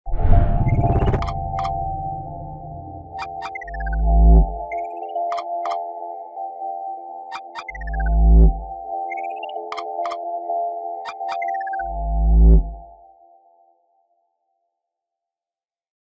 Salomon Alphacross 4 - 3D Motion Video | Cinema 4D | Octane Render
3D motion video displaying the features of a hiking shoe